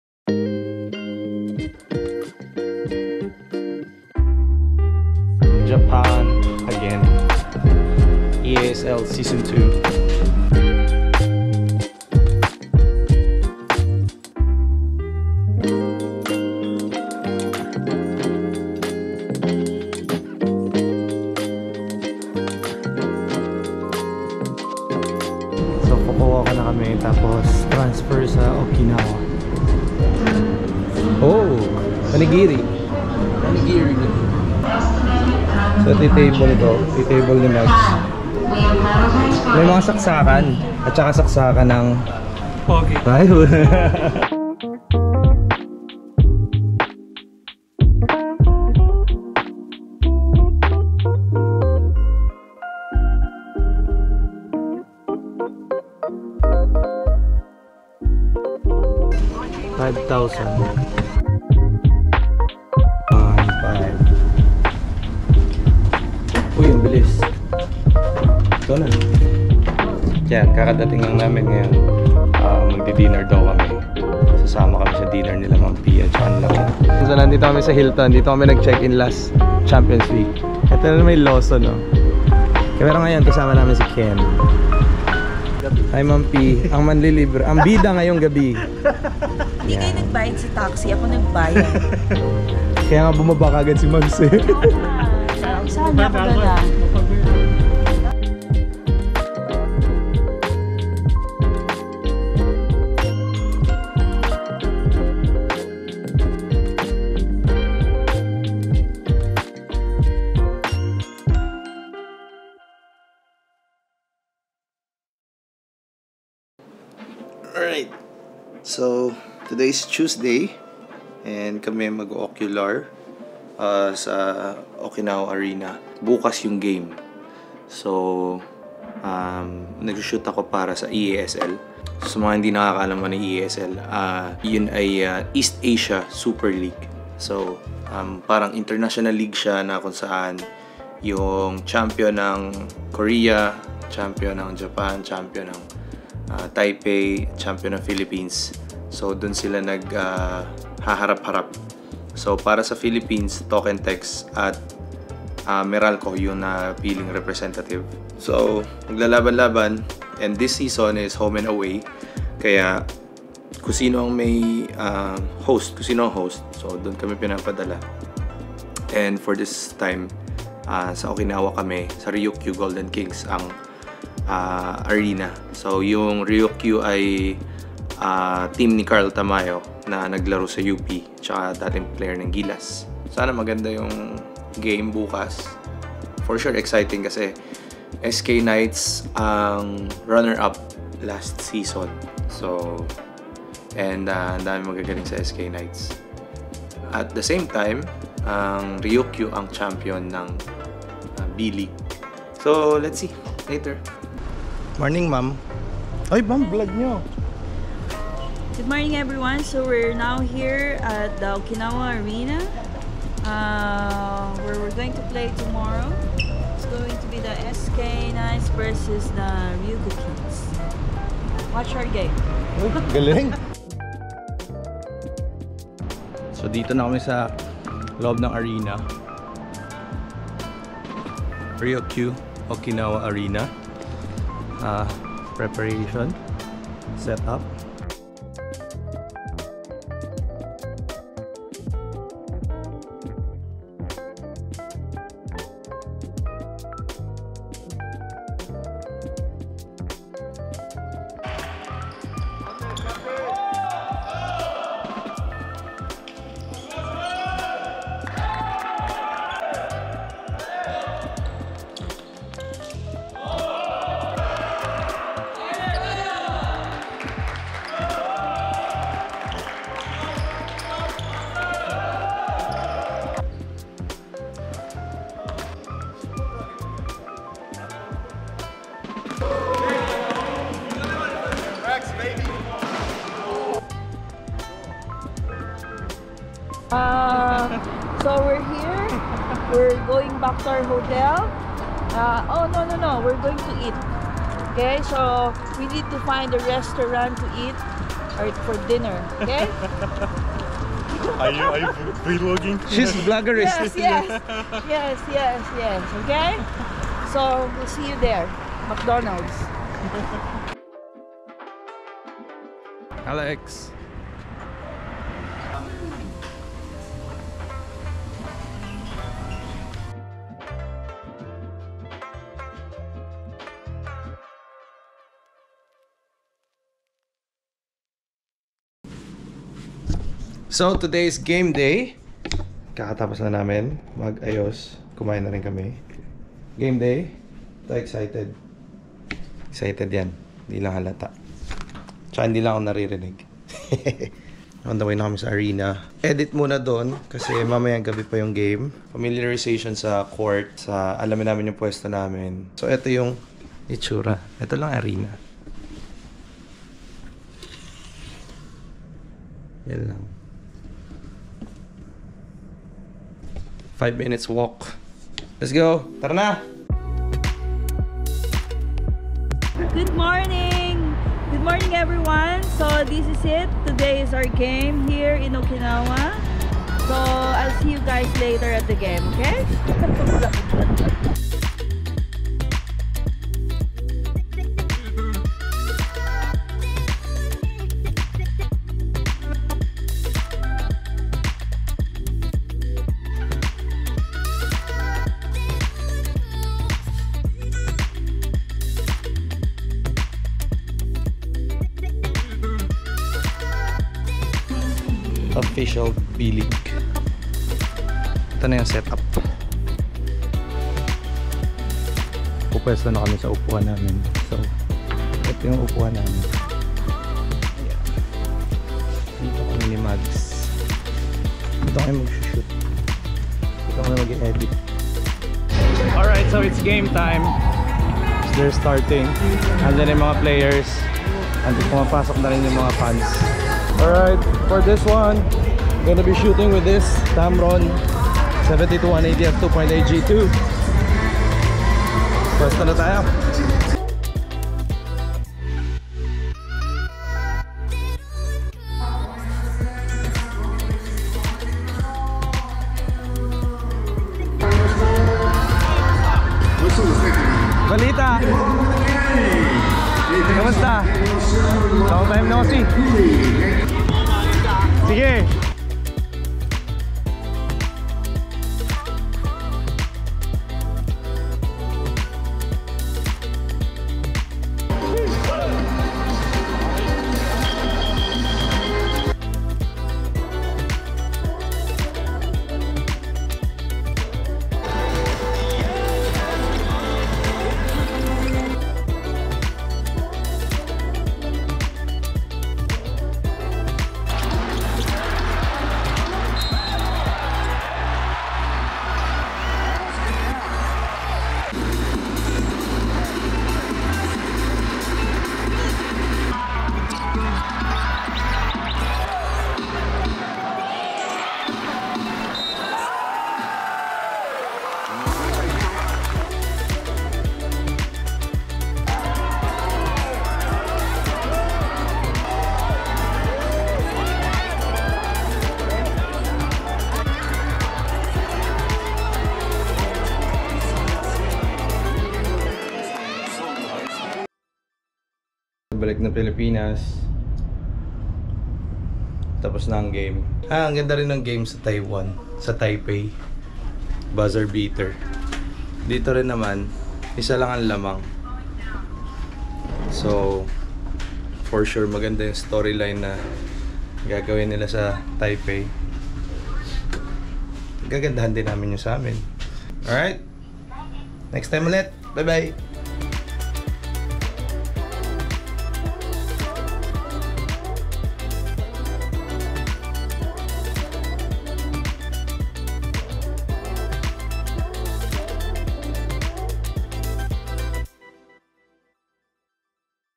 Behind the scenes for the EASL game in Okinawa!
Sharing with you our experience in Okinawa, covering the games for the EASL tournament.